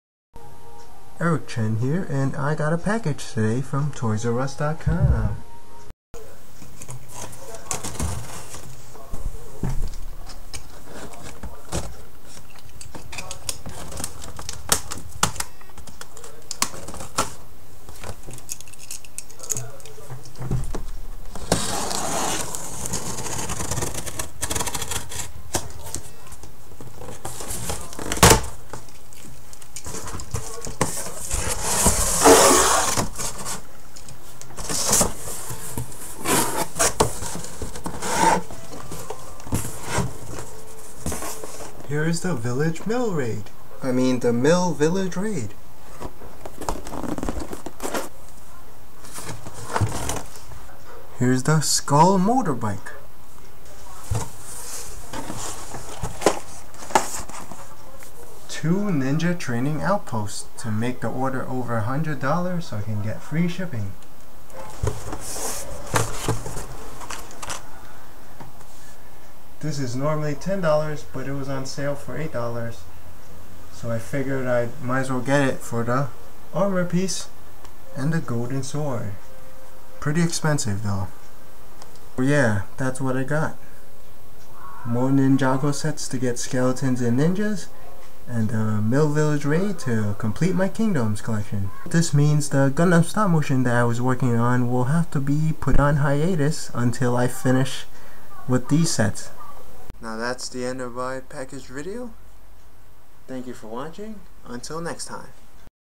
I got a Package #23
Kingdoms Wave 2
7189 Mill Village Raid

Lego Ninjago Wave 1
2516 Ninja Training Outpost
2259 Skull Motorbike
2114 Chopov Spinner